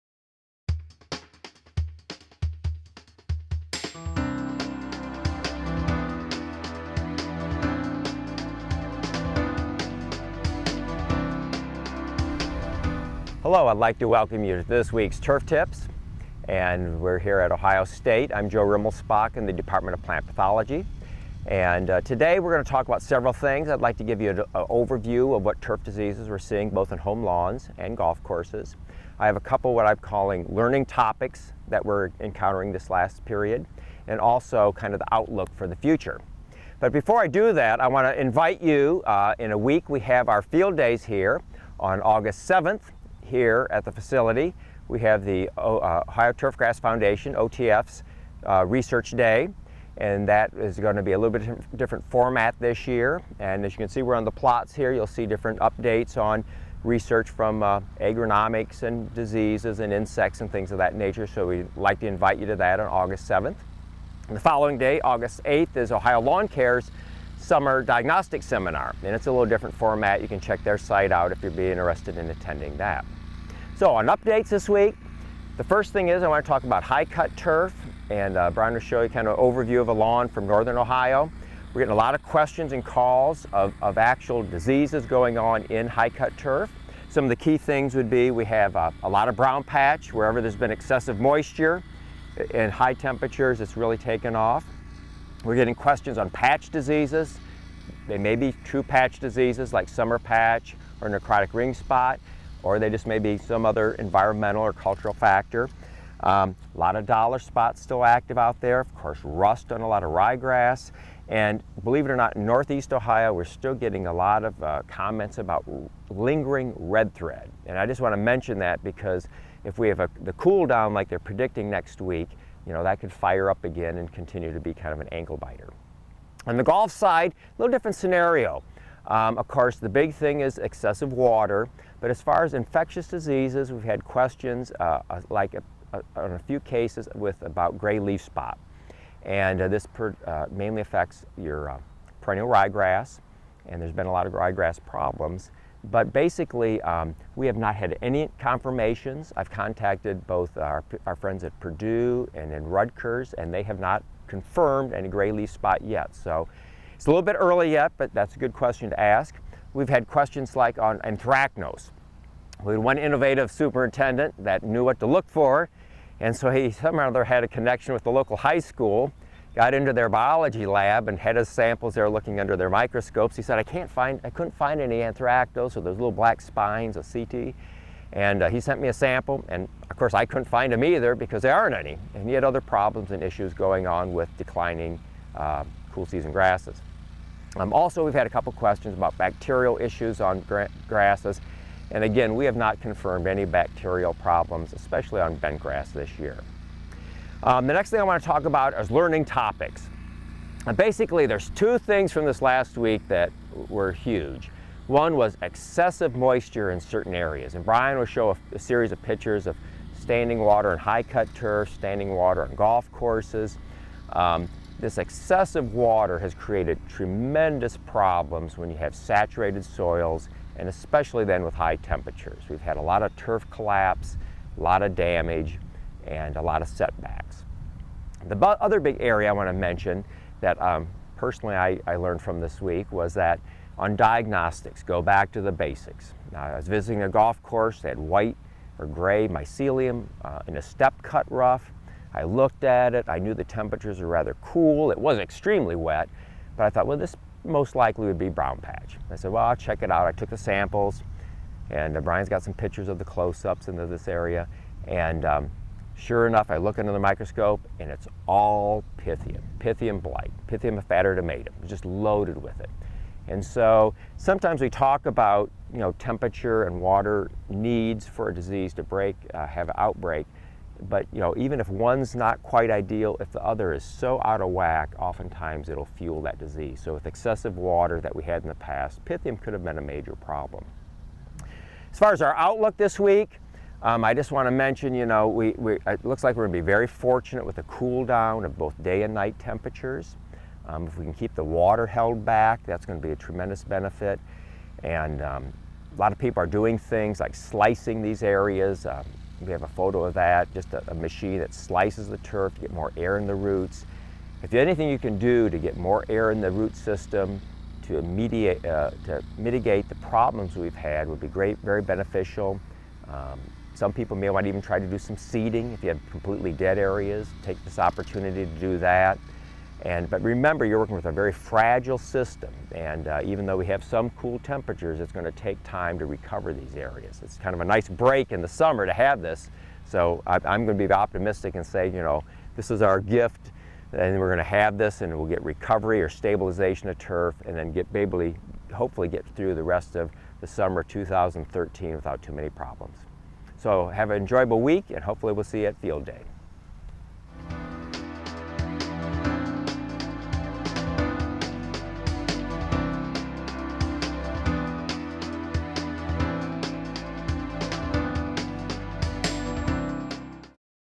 Turf Tips - July 26, 2013
Joe Rimelspach from OSU Plant Pathology discusses the lastest diseases impacting turf in Ohio and the surrounding region. This week, Joe discusses confirmed cases of pythium, red thread and more.